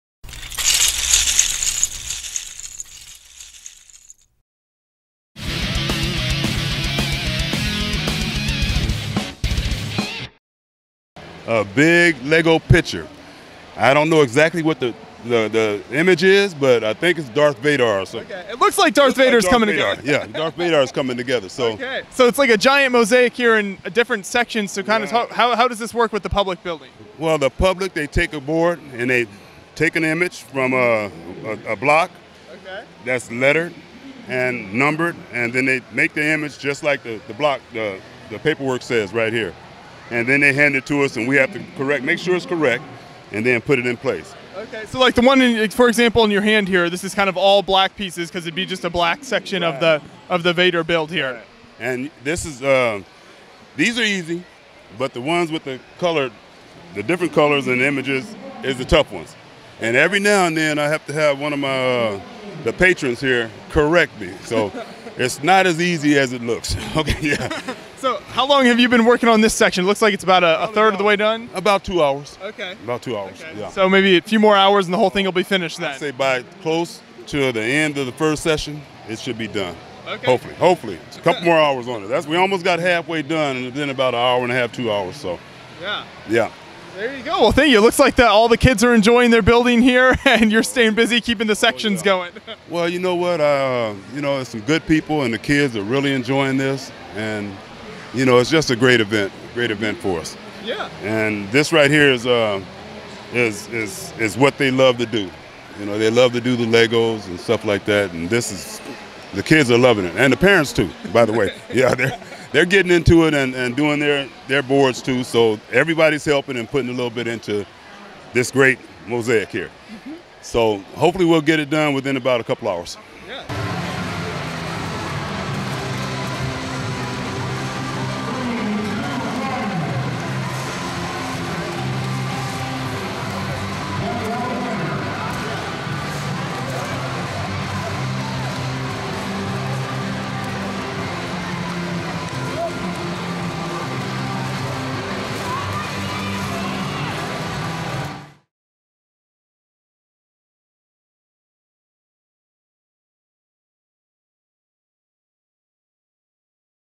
Creating a Gigantic LEGO Star Wars Mosaic at Brick Fest Live Phoenix
Beyond the Brick's Joshua Hanlon checks out the giant LEGO Star Wars Darth Vader mosaic at Brick Fest Live Phoenix 2017.